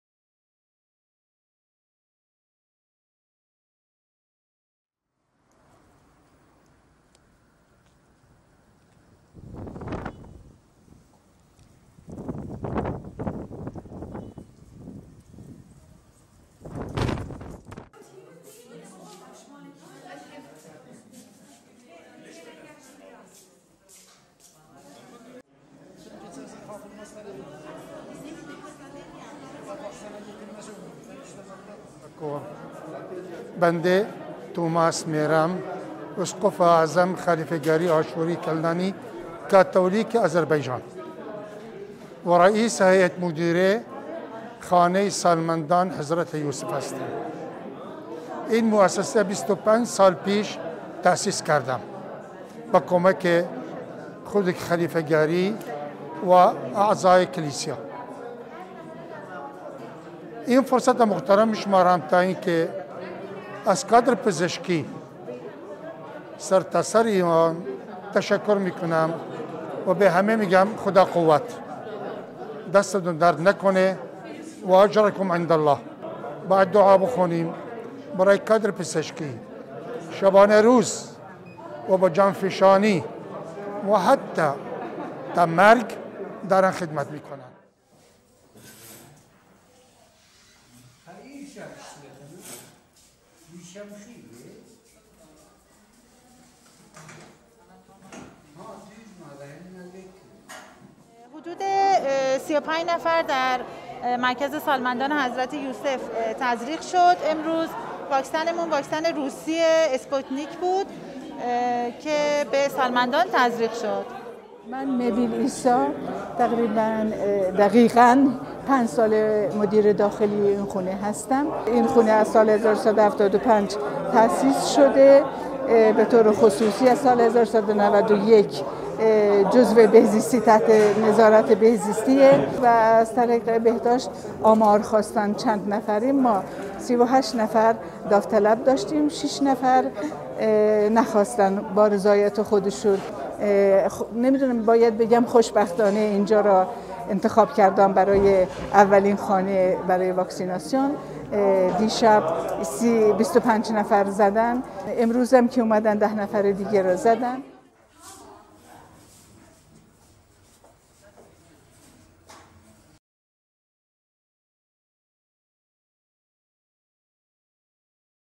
واکسیناسیون سالمندان مسیحی در ارومیه
واکسیناسیون سالمندان استان آذربایجان غربی، با تزریق واکسن کووید-۱۹ به سالمندان مسیحی ساکن در سرای حضرت یوسف (ع) ارومیه آغاز شد. تاکنون ۹ هزار دوز واکسن کرونا به استان آذربایجان غربی ارسال شده که تزریق نیمی از آن‌ها انجام شده است.